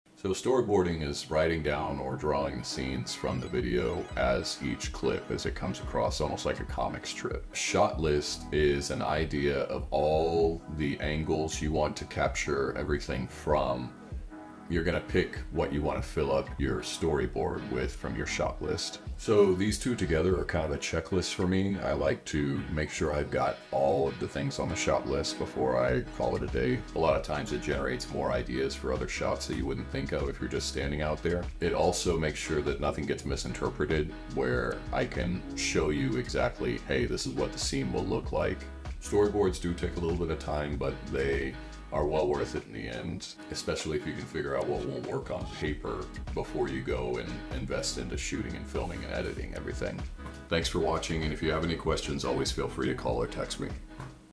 4 Storyboard- just plan better to save money and time
I go over what a story board is and its role in making a better video.